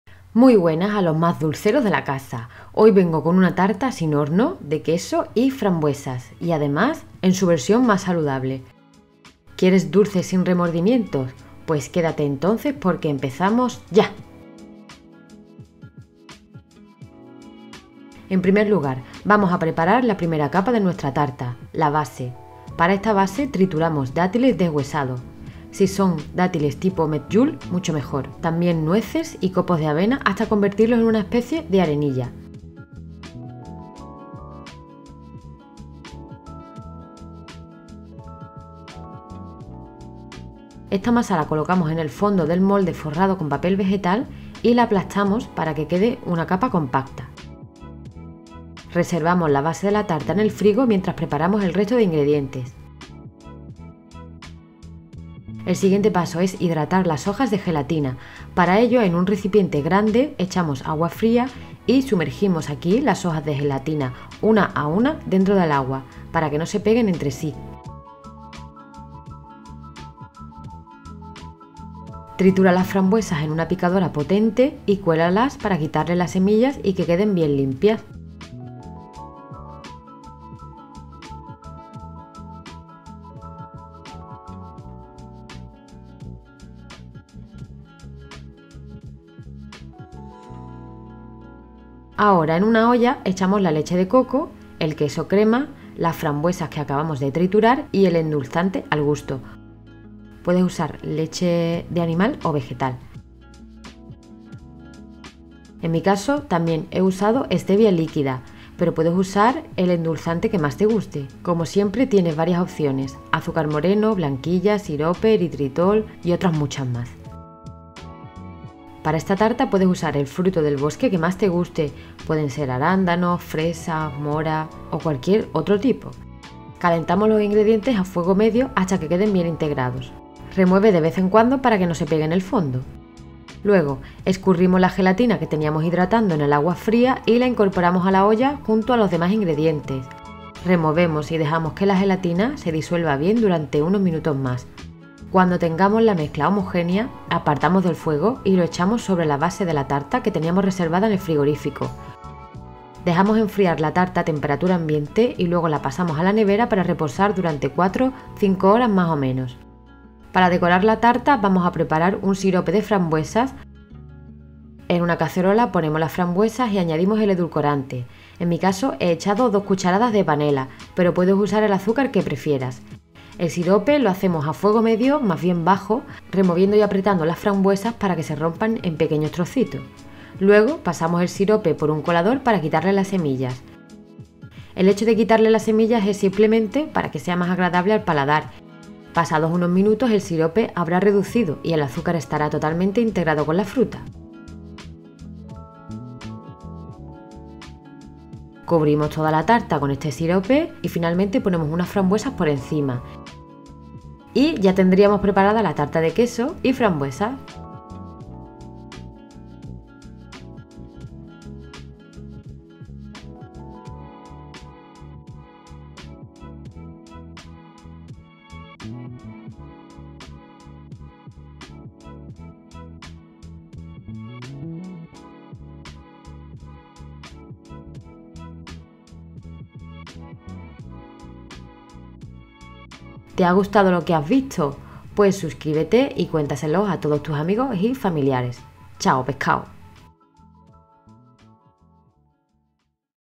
TARTA de Queso y Frambuesas SALUDABLE 😋
Aquí te dejo una tarta de queso y frambuesas en su versión más saludable. No olvidemos que el azúcar también es importante para nuestro cuerpo y que, tipos de azúcar, hay muchos. Así que elige el que mejor te siente y más te guste :)

Abajo te daré algunas opciones para sustituir ingredientes.

Espero que disfrutes de esta receta.

INGREDIENTES

Para la base:

100 gr. Dátiles deshuesados (Medjoul)
100 gr. Nueces
40 gr. Copos de avena

Para el relleno:

250 gr. Frambuesas
200 gr. Queso crema light
300 ml. Leche de coco
10 gotas de estevia líquida ( o 100 gr. Endulzante)
4 hojas de gelatina

Para el sirope:

150 gr. Frambuesas
3 gotas de estevia líquida o 2 cucharadas de panela

ÍNDICE DE ESTA RECETA

0:00 👋🏻 Intro receta + Ingredientes
0:13 1️⃣ Base de la tarta
0:55 2️⃣ Cómo se prepara el relleno
2:41 3️⃣ Receta del sirope
3:40 🍽 Emplatado y resultado final

🍽 MÁS RECETAS DE TARTAS SALUDABLES  🍽

🔔¿Prefieres ver recetas en menos de 1 minuto? VISITA mi CANAL de RECETAS CORTAS Aquí👉🏻